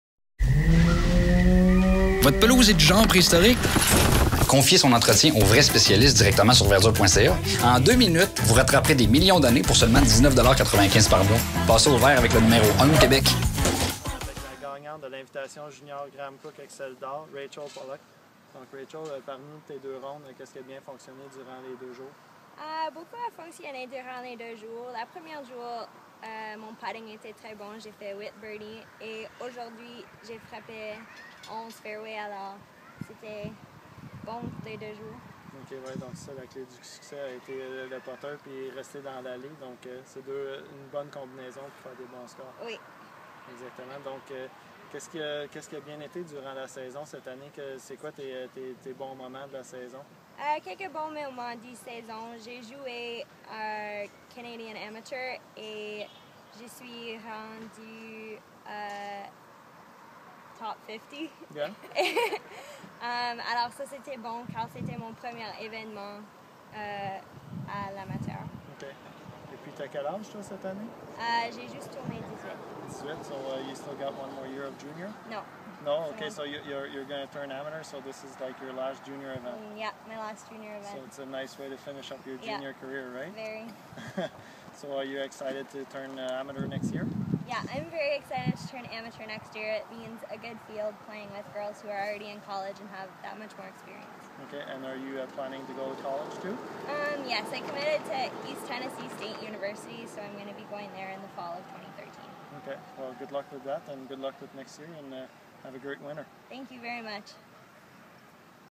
Rachel Pollock - Championne de l'Invitation junior Graham Cooke Exceldor
Eric Couture s'entretient avec Rachel Pollock du club de golf Cutten Field suite à sa victoire lors de l'Invitation junior Gaham Cooke Exceldor présentée par Vertdure qui s'est déroulé au club de golf Château Bromont les 25 et 26 août 2012.